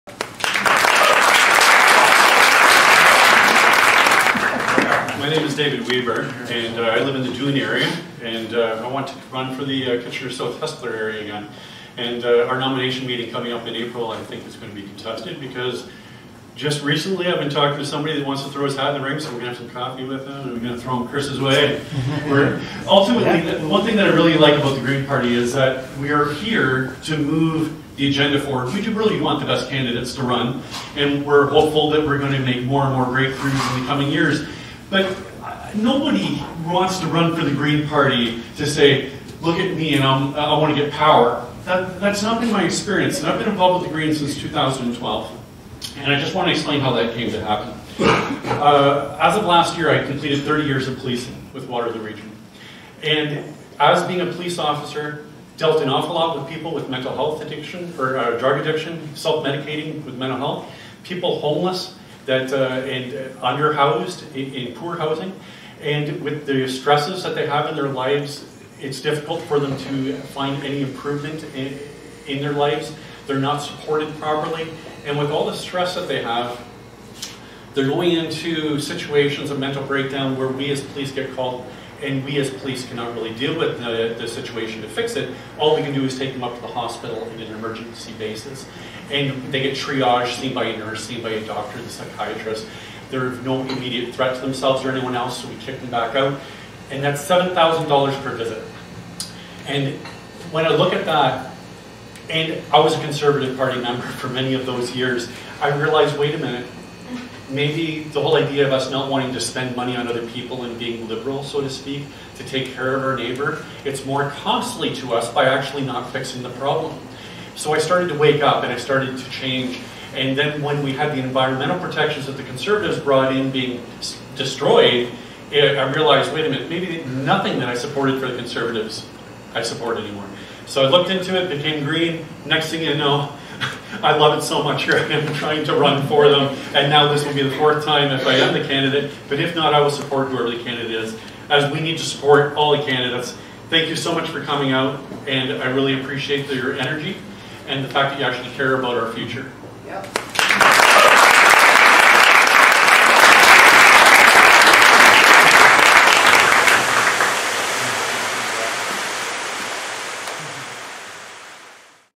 David Weber for Kitchener South—Hespeler
Our 2019 WRGreens Candidate Nominees were asked:

"Running for office is hard, thankless work.  What made you do it?"

Video recorded at the 2019 WRGreens Candidate Nomination event held on Thursday, January 31st, 2019 at Fresh Ground, in Kitchener